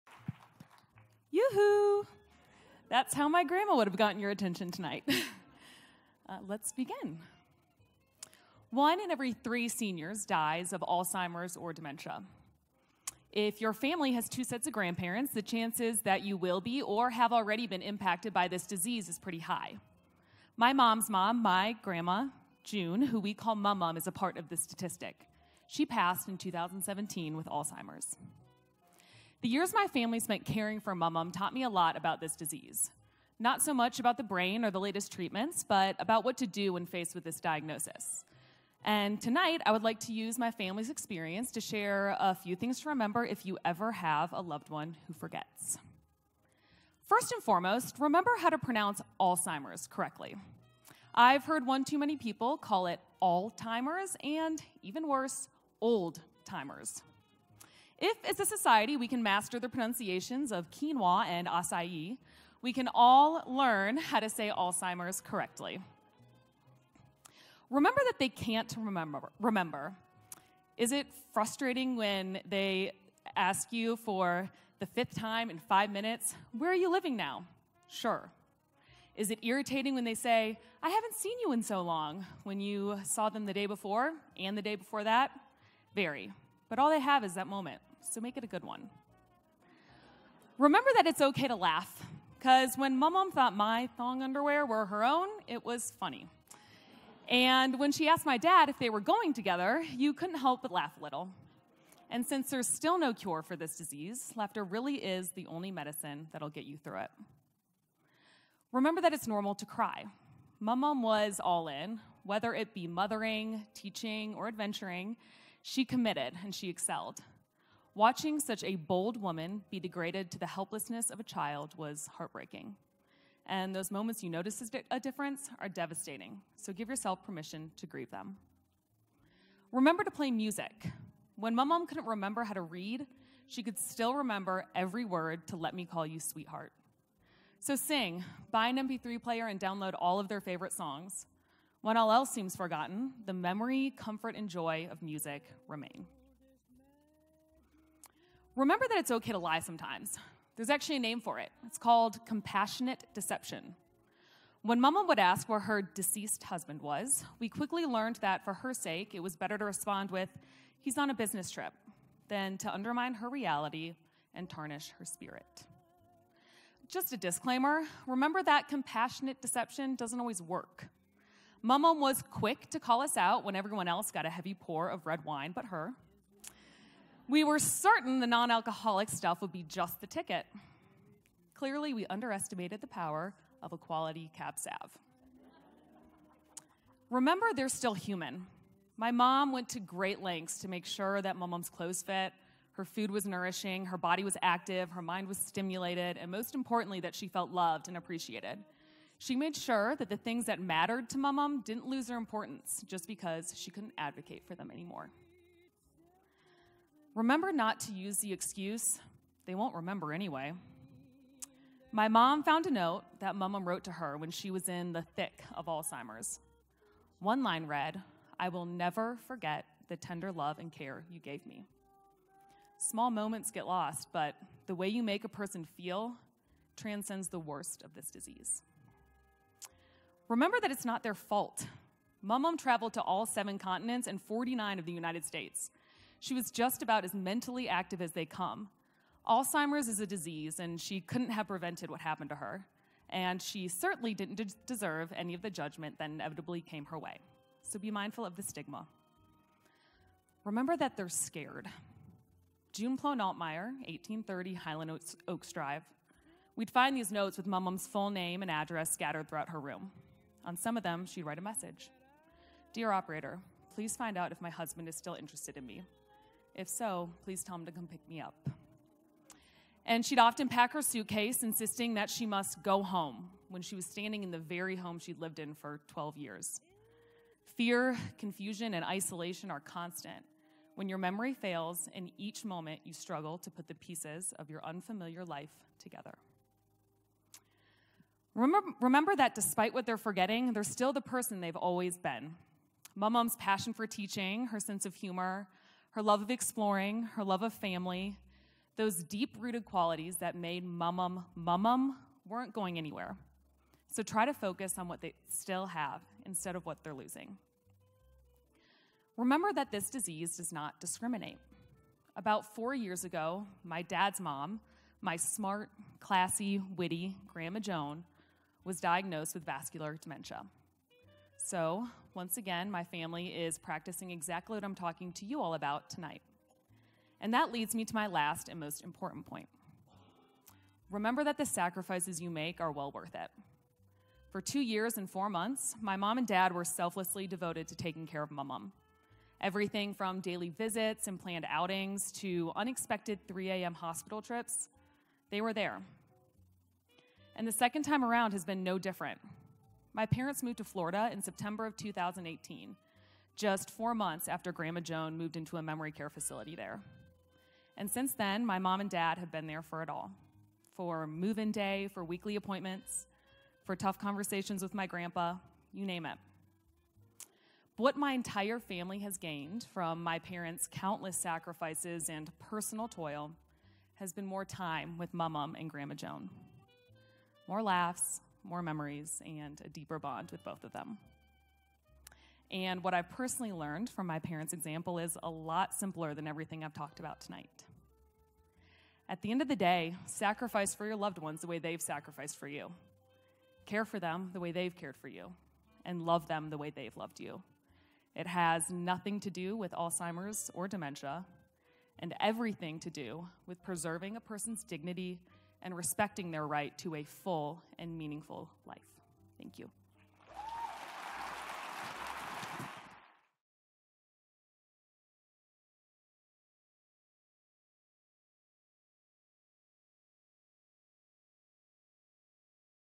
Liddy Stewart - What to Remember When Your Loved One Forgets
Bozeman Pecha-Kucha Night, February 20-21, 2019